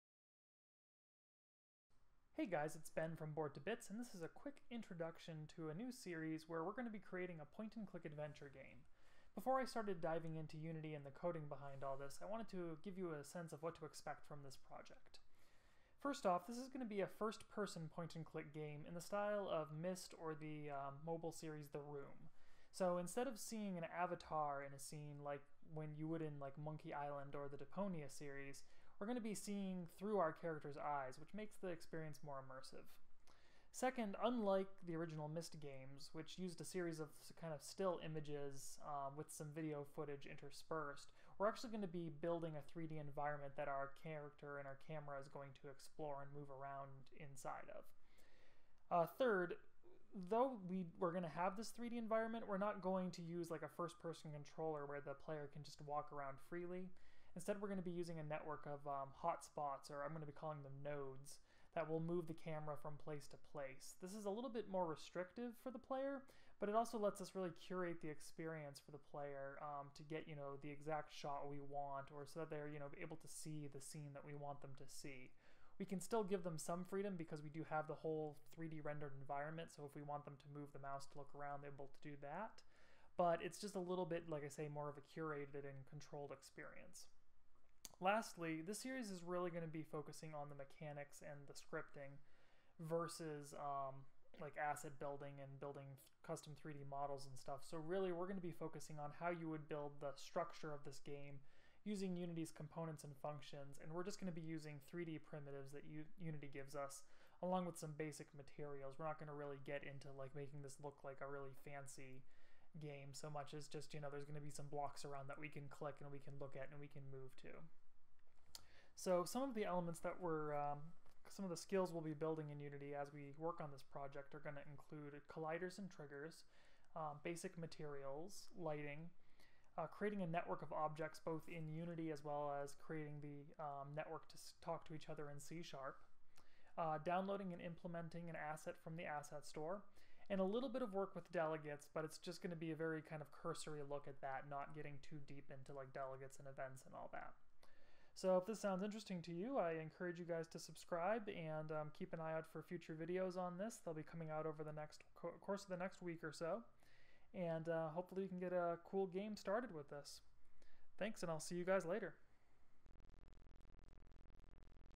Unity Point-and-Click Game Tutorial: Introduction
Quick overview of a series on building a first-person point-and-click adventure game in the style of Myst or The Room using Unity.